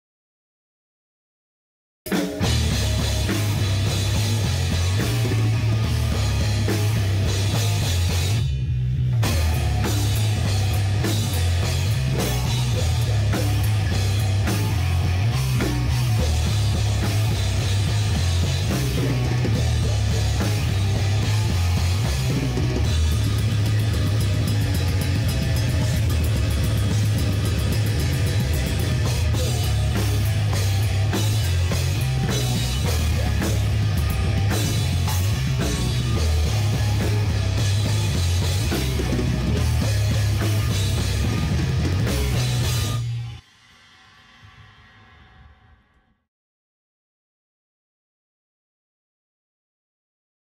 Culled Rehearsal Snippet 04.04.23
Culled Rehearsal 04.04.23 - A snippet of a new track to be recorded soon.

Ben Harbak - Guitar
Chris Chapman - Bass
Curtis Blair - Drums
Shane Hawco - Vocals